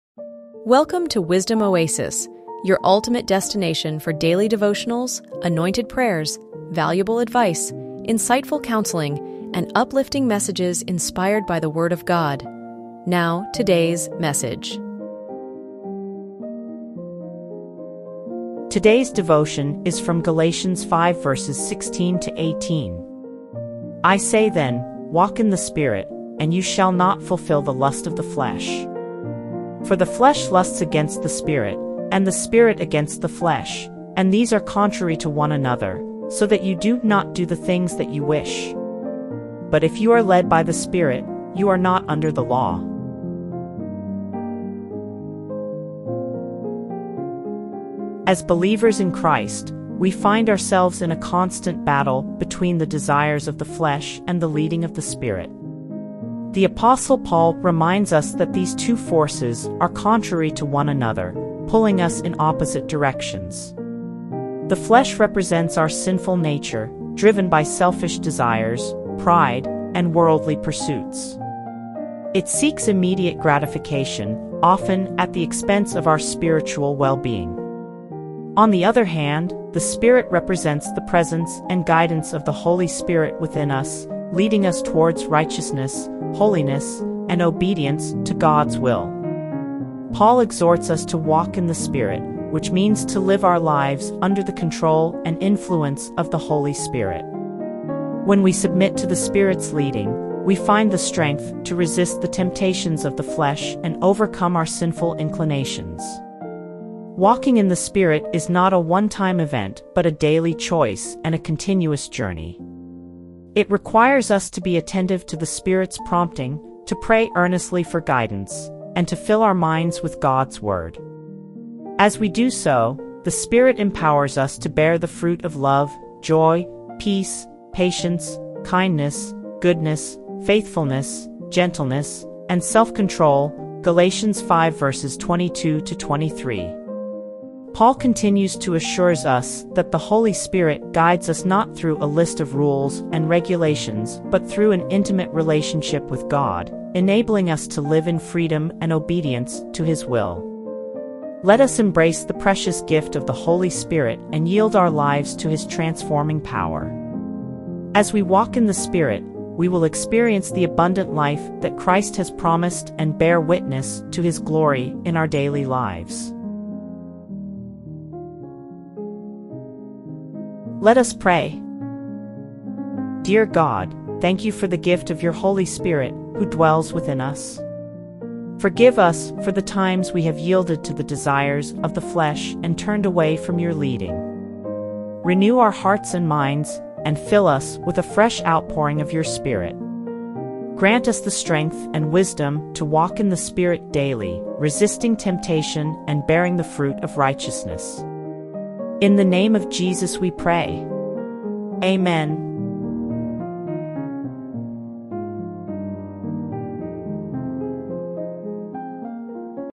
Walking by the Spirit; Galatians 5:16-18 (NKJV) | Daily Devotion. May 04, 2024
Dive into today's devotion and discover the empowering message of Galatians 5:16-18. Explore how "walking by the Spirit" can transform your daily life, helping you overcome the desires of the flesh and align closely with God's will. Embrace the freedom that comes from being led by the Holy Spirit and find clarity amidst life's conflicts. Join us in a prayer to surrender to God's guidance and experience the true liberation that faith brings.